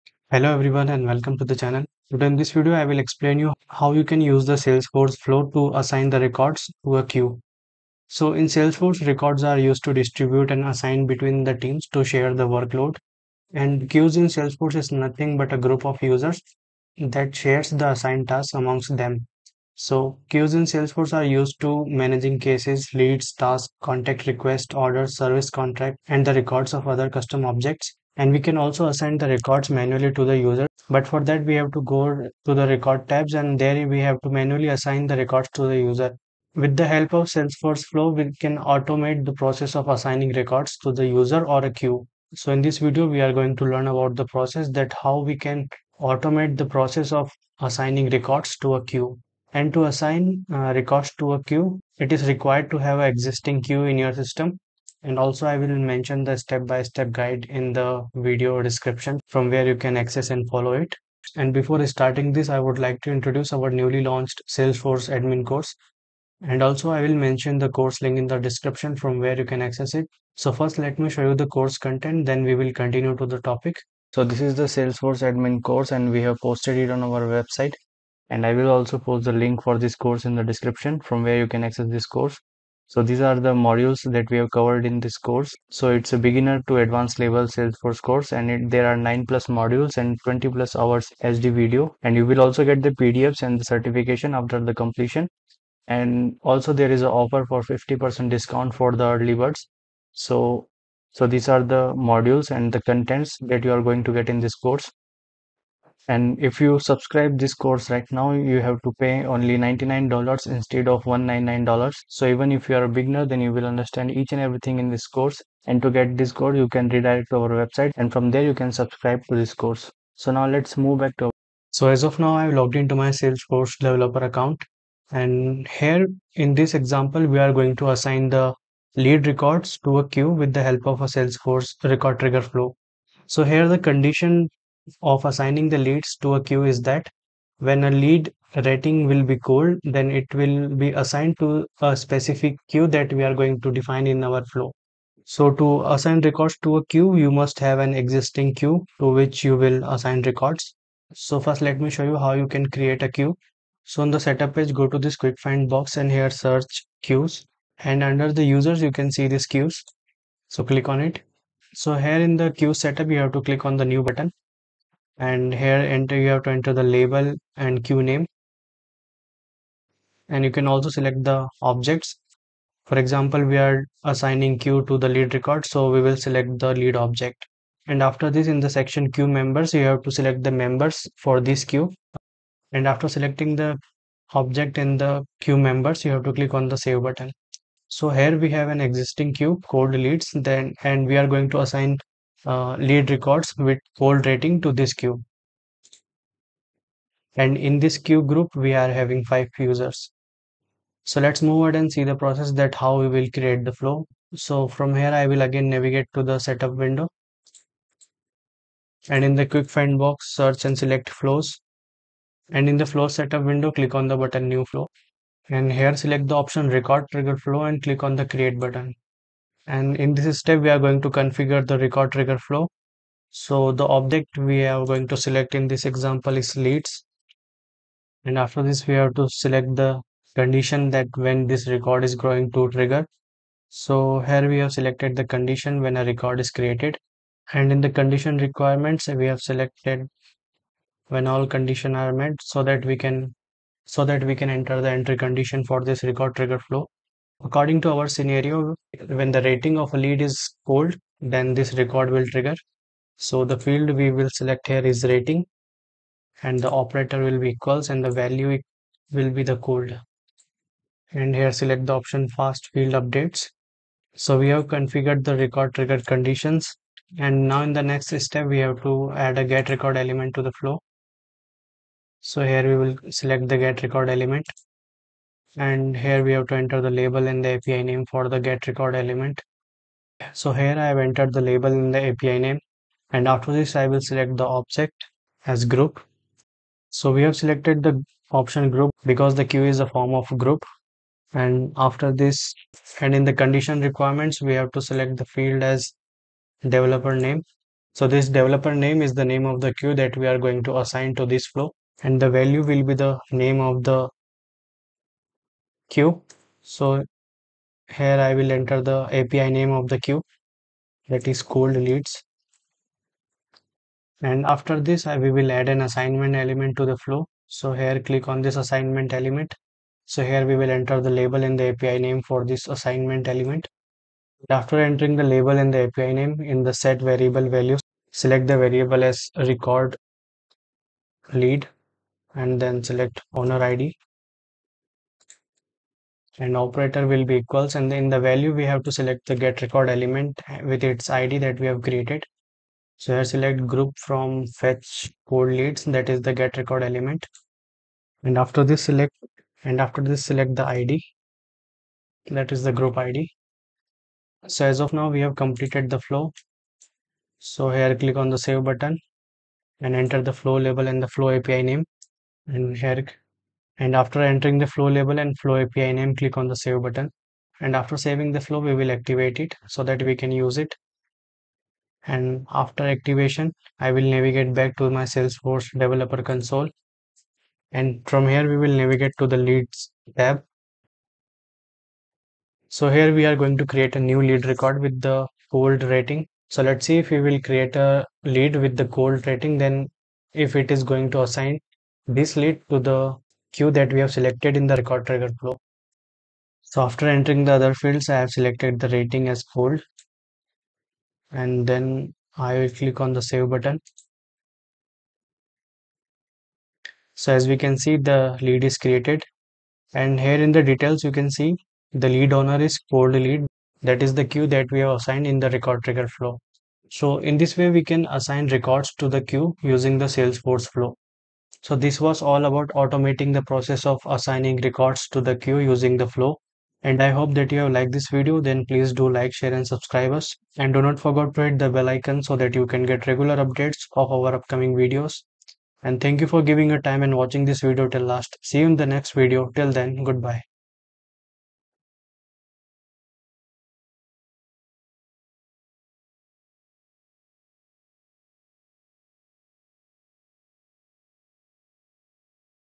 Assign records to queue using flow in Salesforce
In Salesforce, Records are used to distribute and assign records used by teams that share workloads. Queues in Salesforce are used for managing cases, leads, tasks, contact requests, orders, service contracts, knowledge articles, and custom objects.
In this video, I will explain how to Assign records to the queue using the Record trigger flow in Salesforce.
Check out the complete tutorial on How to Assign Records to Queue Using Salesforce Flow